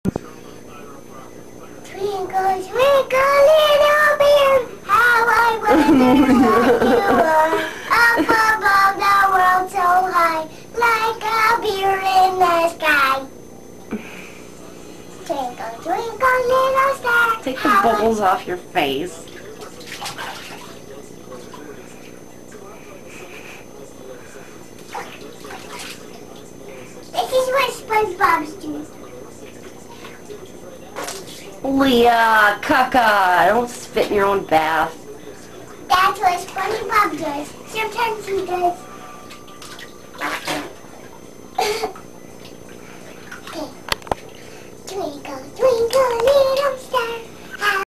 My daughter' Leah showin off her ball of sunshine self while bathing :)